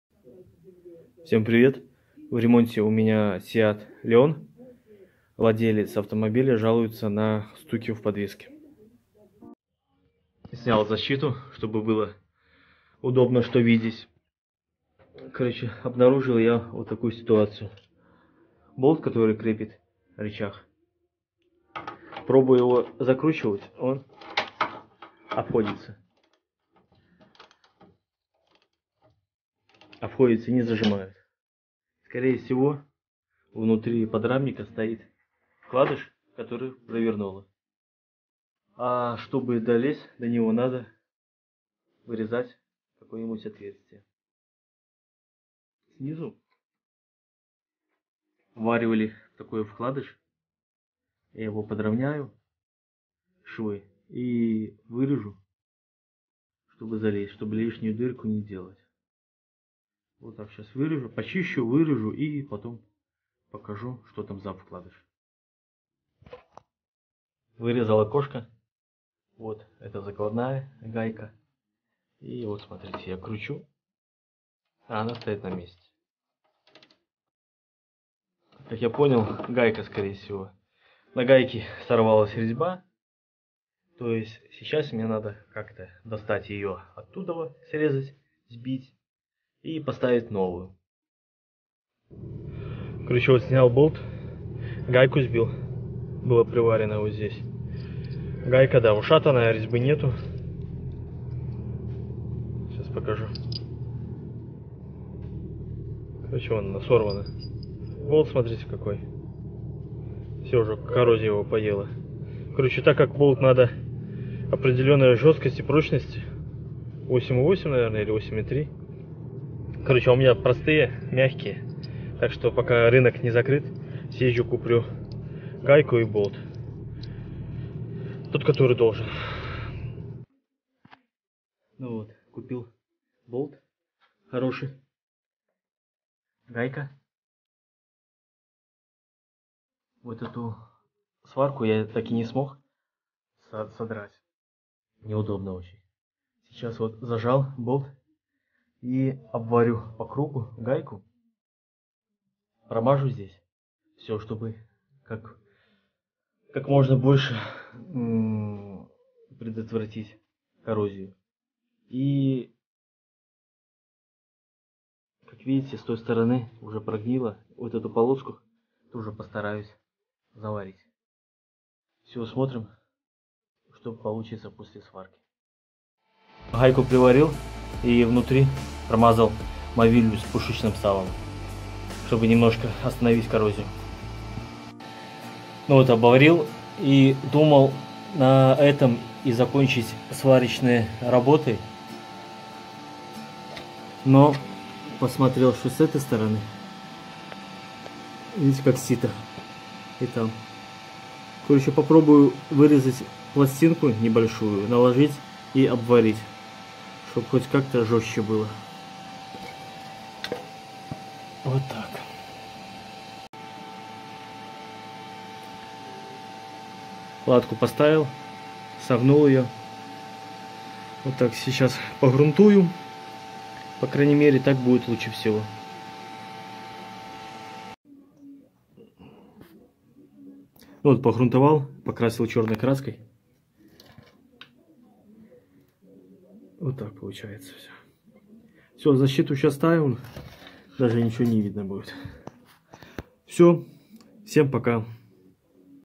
подвеска, провернула болт Seat leon
провернула болт на рычаге сеат леон
підвіска, провернула болт Seat leon